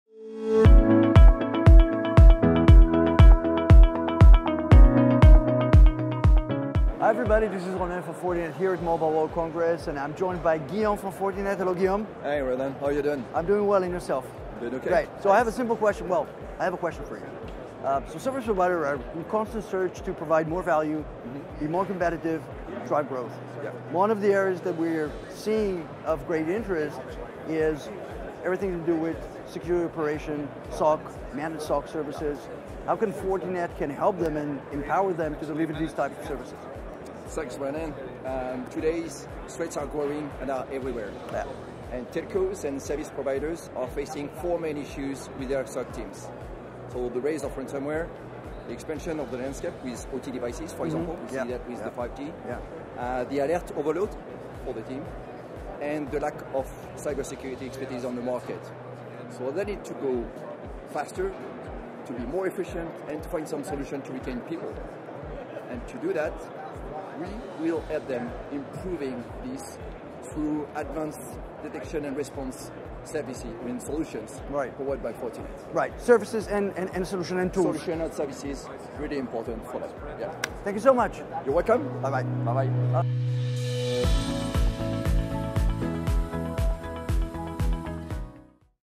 How Service Providers Can Better Protect and Manage Their Networks | MWC23
Ronen Shpirer and Guillaume Schlienger discussed how Fortinet helps Service Providers deliver value through advanced incident monitoring, detection, and response capabilities and services during Mobile World Congress 2023 in Barcelona.